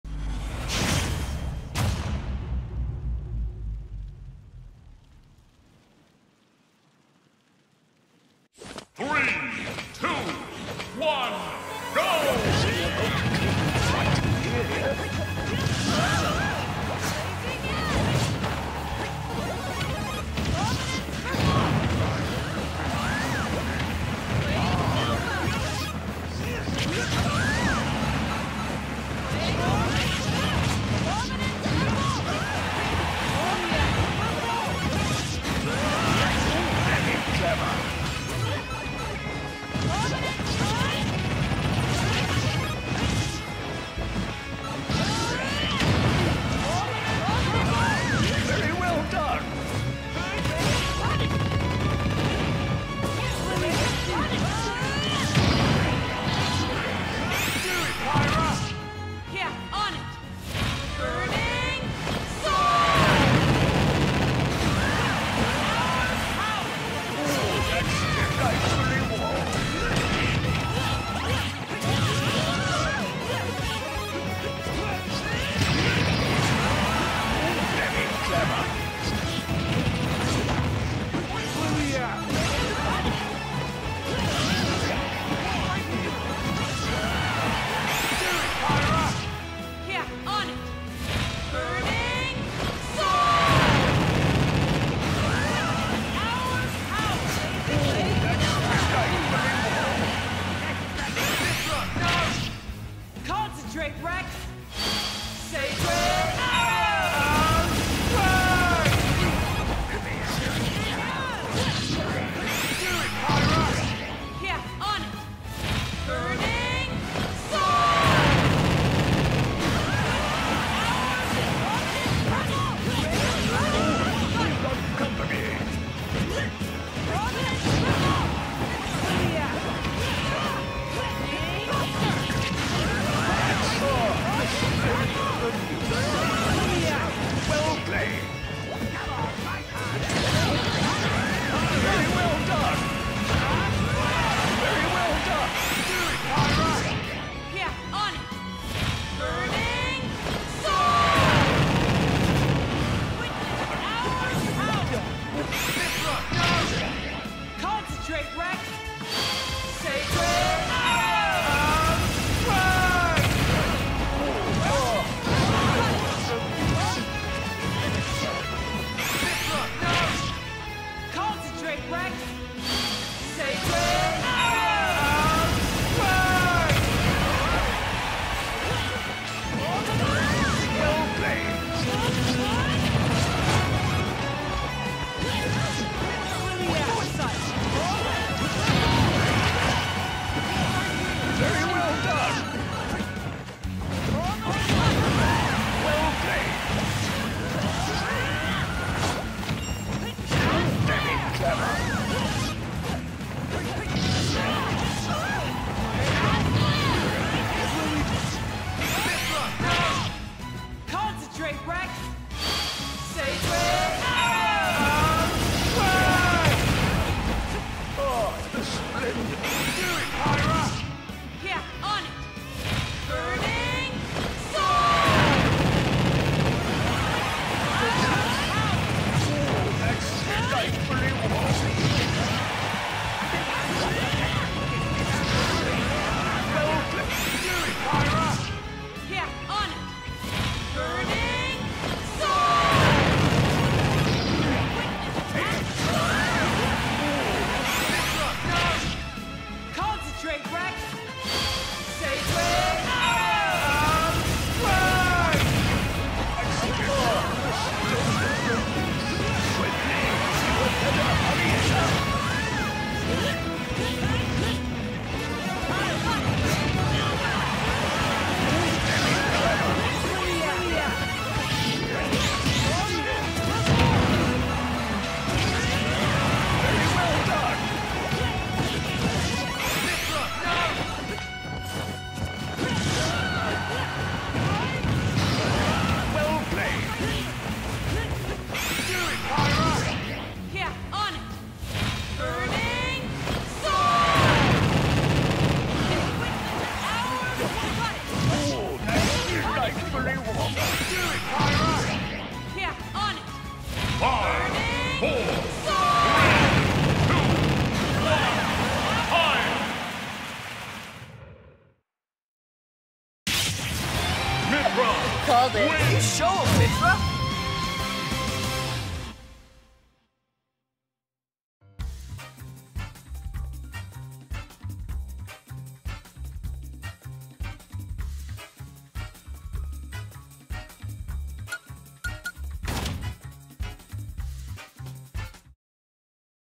Pyra/Mythra Mod Battle
Good Morning everyone.  I will be back tonight with a Livestream again but for today's videos we still just have the match ups chosen by me.  First up is a 4 way Pyra/Mythra Mod Battle.

Dark Stalkers vs Naughty Nurses vs Aegis SIsters vs Sailor Scouts.